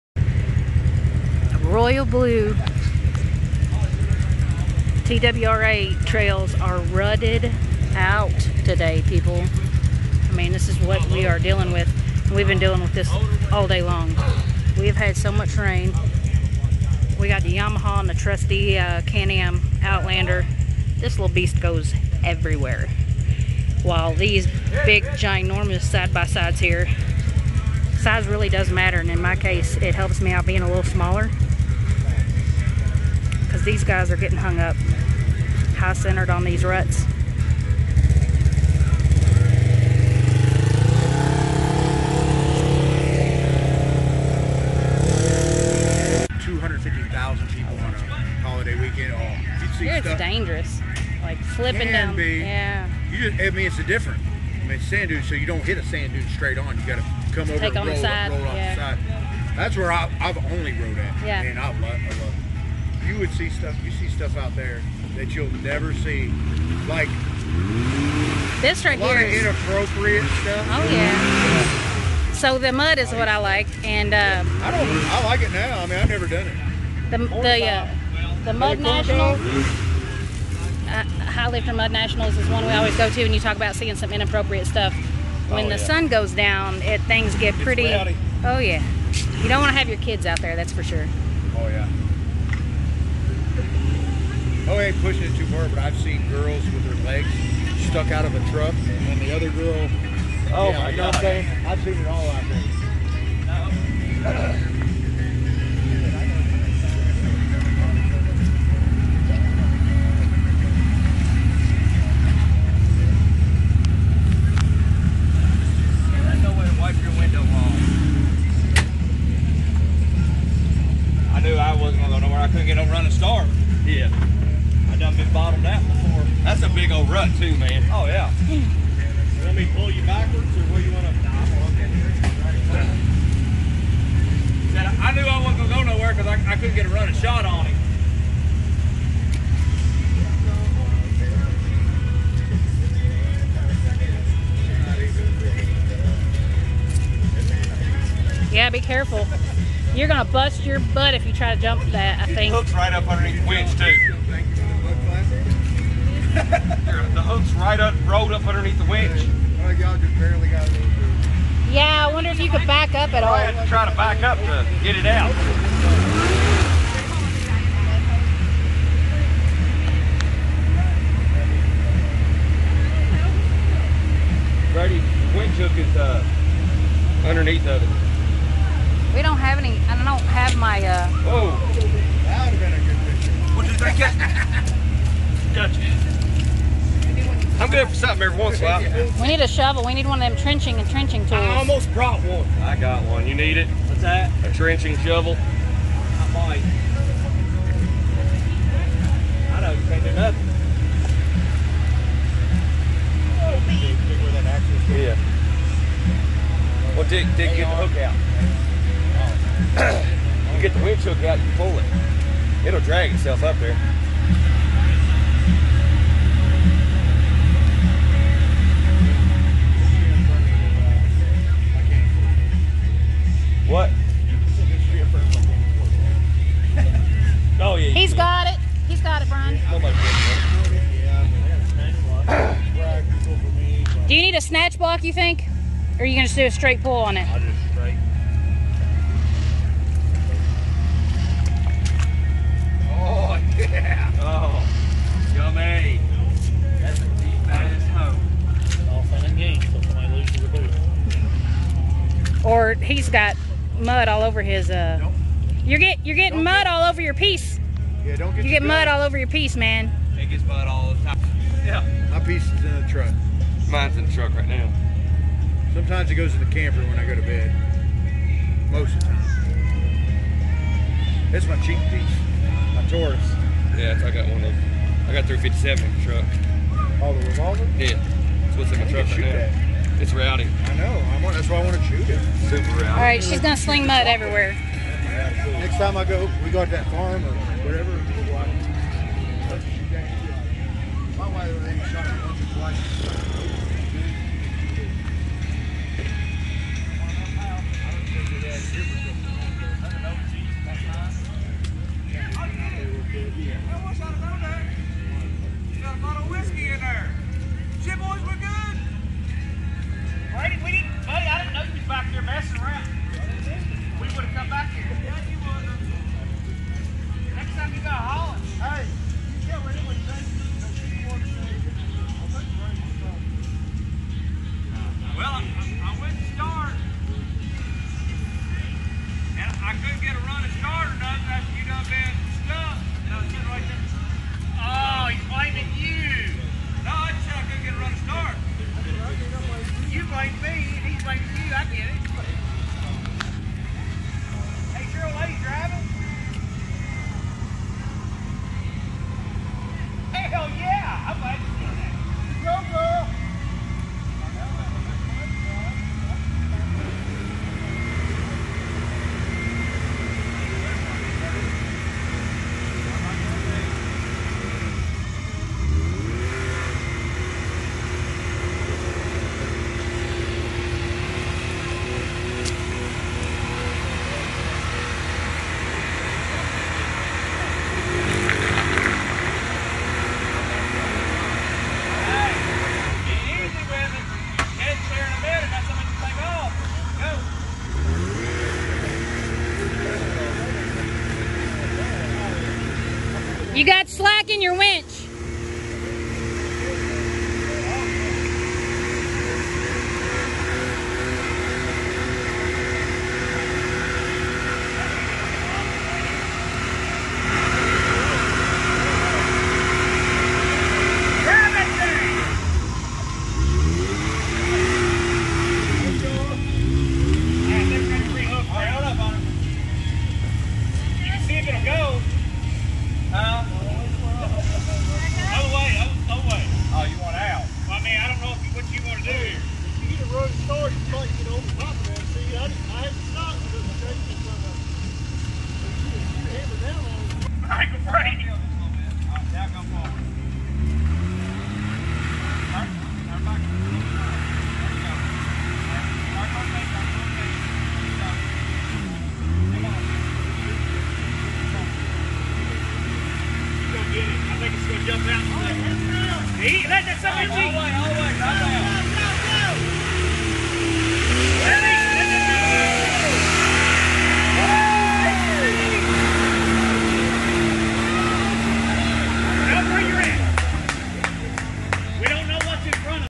The ruts & mud were hell at Royal Blue/TWRA!
We had to break out the winch a few times this trip...LOL!